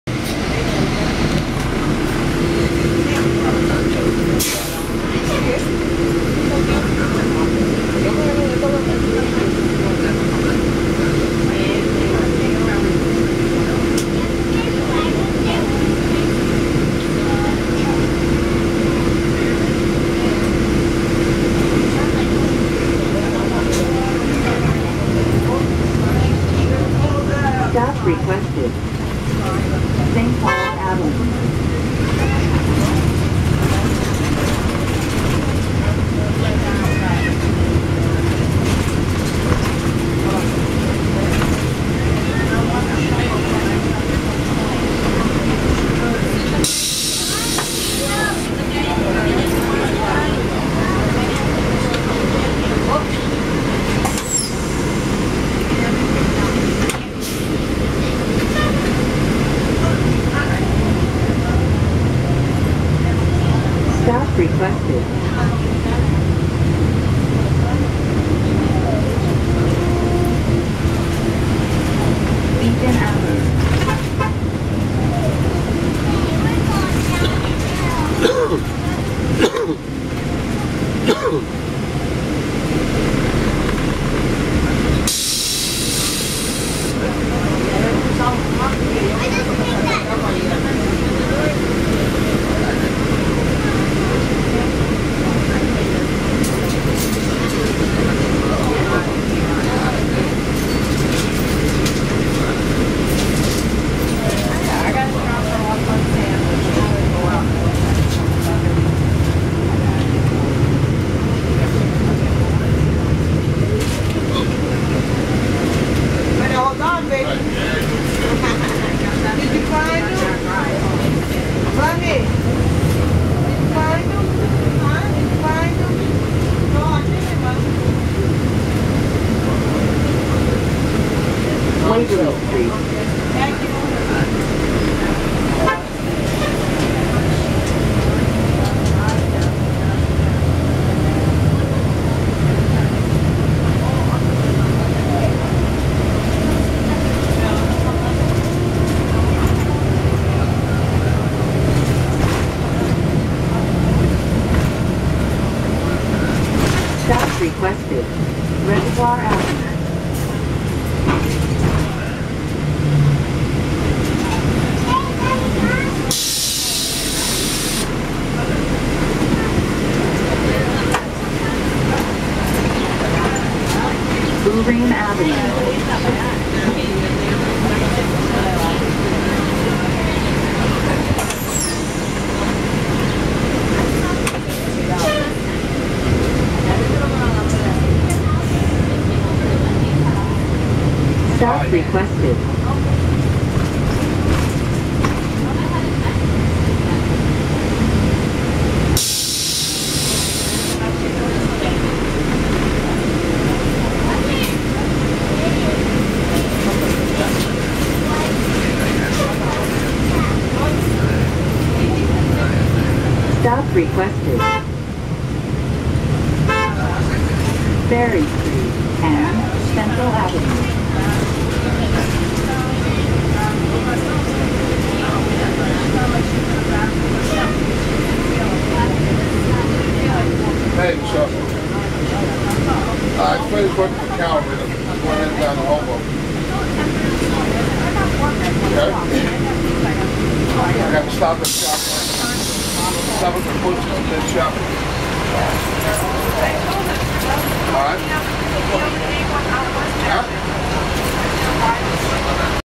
NJT NABI 40-SFW #5260 on the 87 to Hoboken (Inside, 3/4)
Here's a ride on-board NJT 2009 (North American Bus Industries) NABI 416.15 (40-SFW) #5260 on the 87 to Hoboken-Path via Journal Square on a Saturday Afternoon;  this bus wasn't as packed after Journal Square; this is part 3 of 4.